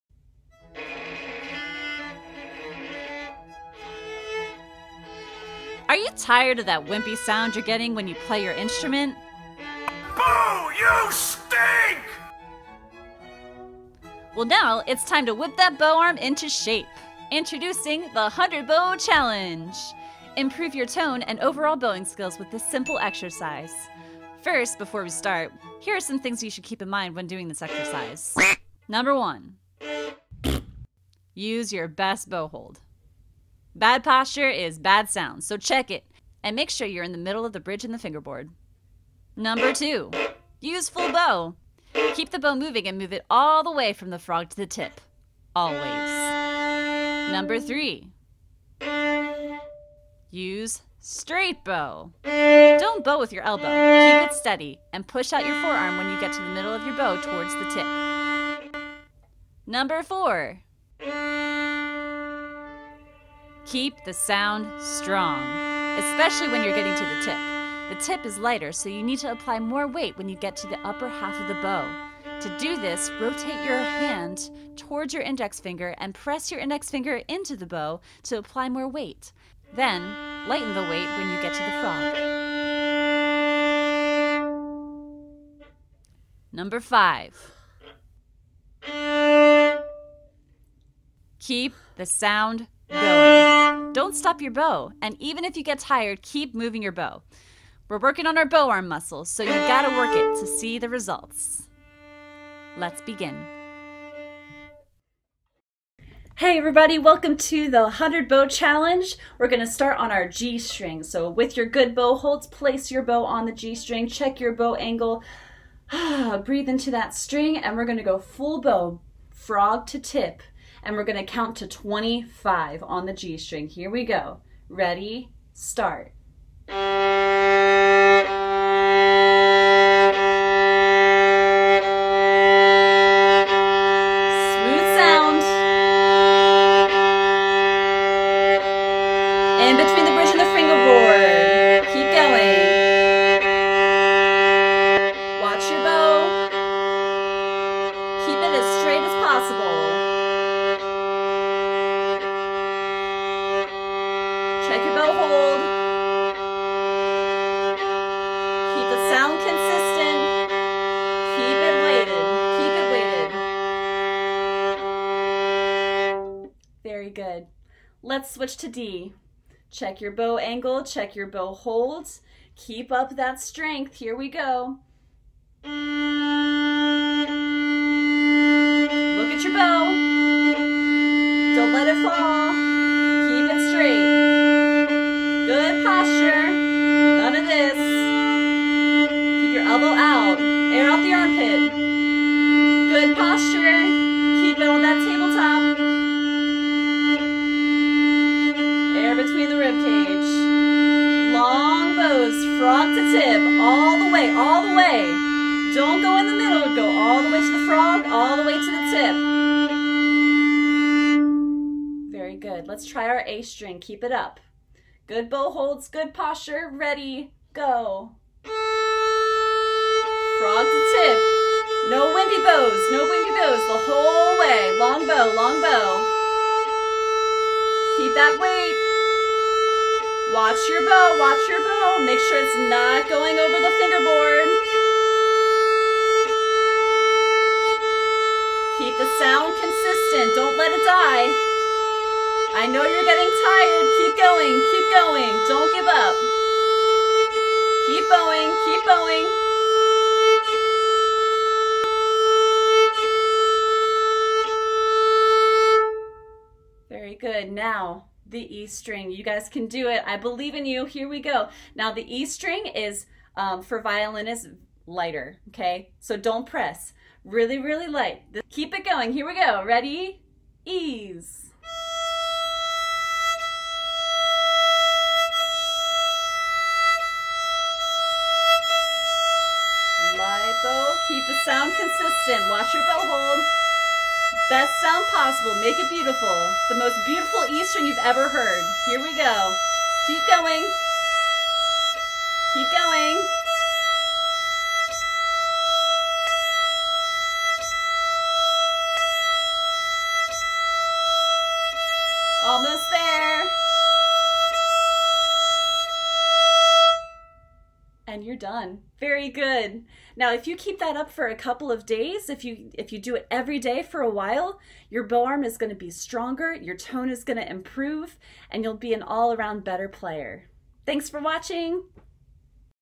100 Bow Challenge - Violin and Bass Strings
In this exercise we will play 25 bow strokes on each of the four strings, practicing good posture, technique and sound.